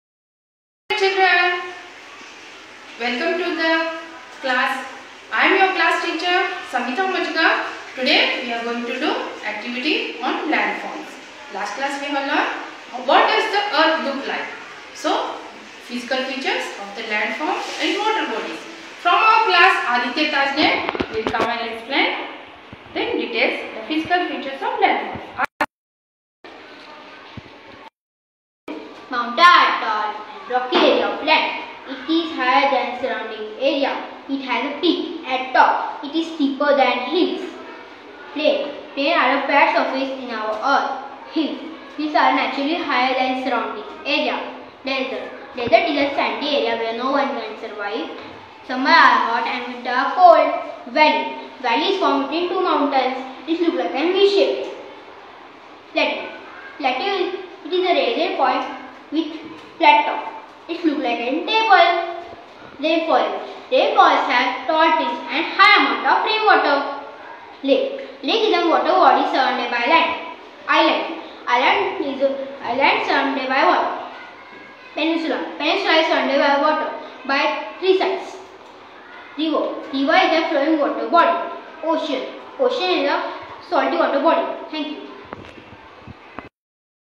Activity on Landform | EVS | student explain about landform...
Class 4th EVS activity on landforms by Tr. Savita